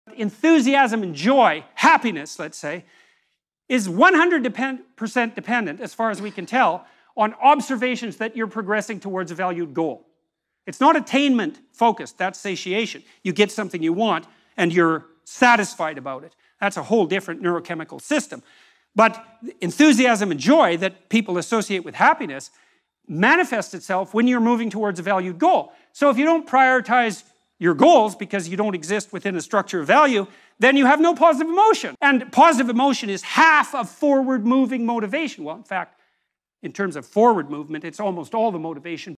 You Will never be Happy Unless You Have This | Jordan Peterson
This clip is from a lecture of Dr. Jordan Peterson. In this clip Dr. Peterson explains that the only way to experience positive emotion (happiness, joy...) is to have the feeling that you are progressing toward a valued goal. This means that you will never be happy unless you have a goal and try to pursue it